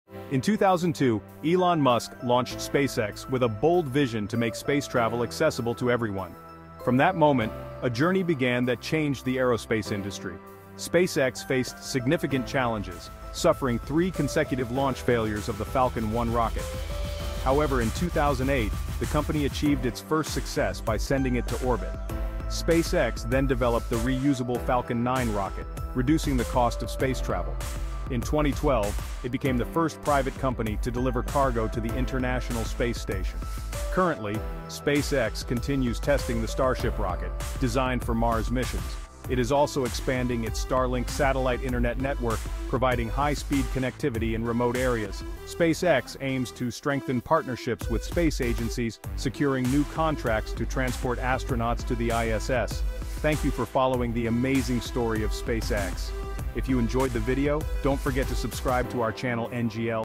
The Amazing Journey of SpaceX: From Failure to Mars!
Discover the incredible story of SpaceX, the company that revolutionized space travel. Founded by Elon Musk in 2002, SpaceX faced early challenges but achieved groundbreaking successes, including reusable rockets and missions to the International Space Station. Learn about the ambitious Starship project and the expanding Starlink network that’s connecting the world. Join us as we explore how SpaceX is paving the way for humanity's future in space exploration!